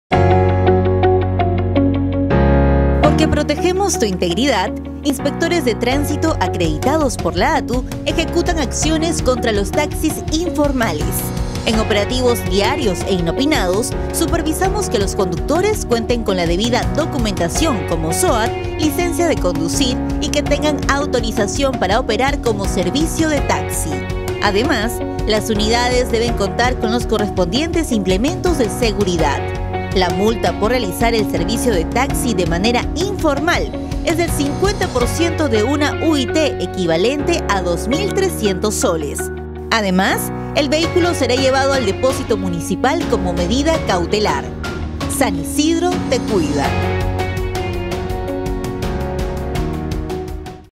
Operativos inopinados de fiscalización a conductores y vehículos.
Preocupados por el bienestar de nuestra comunidad, realizamos constantemente operativos inopinados de fiscalización a conductores y vehículos que brindan servicio de taxi, verificando  los implementos de seguridad y la debida documentación como licencia de conducir, soat, entre otros.
Alcalde:  Augusto Cáceres Viñas